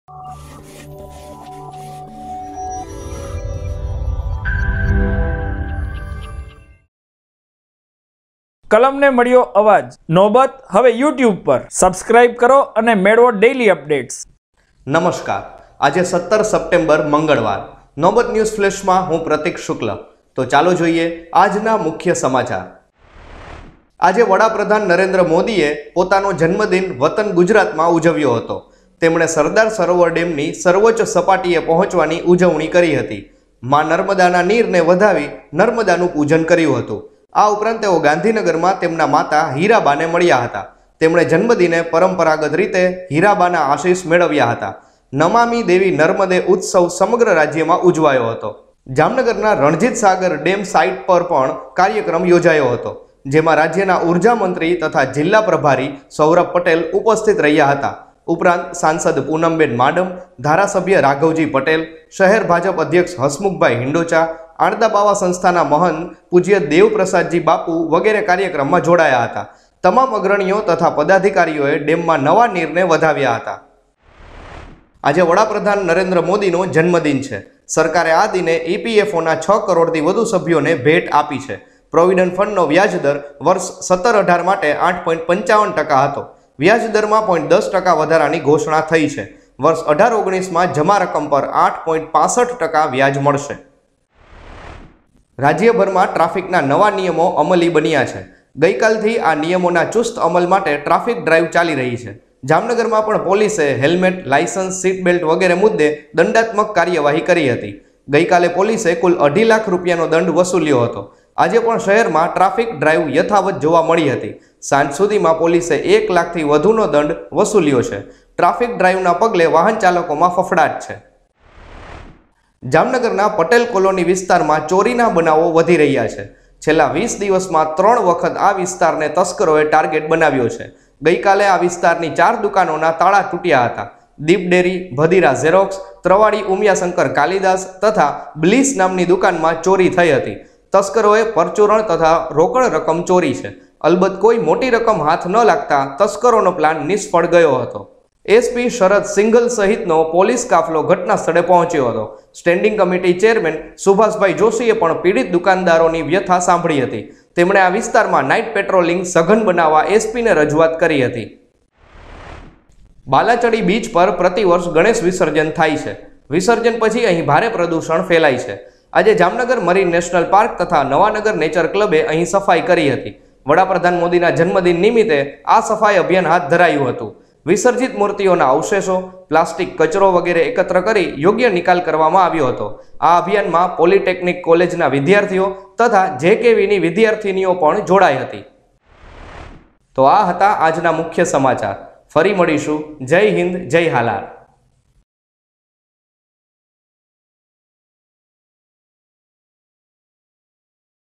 News Flash: 'Namami Devi Narmade' Celebrated in Jamnagar on PM Modi's Birth Anniversary.
1. 'Namami Devi Narmade' Celebrated in Jamnagar on PM Modi's Birth Anniversary.
2.  0.10 % Hike in PF Interest on PM's Birthday.
3. Day 2 of Traffic Drive in Jamnagar.
4. Four Shops looted in Patel colony area of Jamnagar.
5. Cleanliness campaign at Balachadi beach by Nawanagar nature club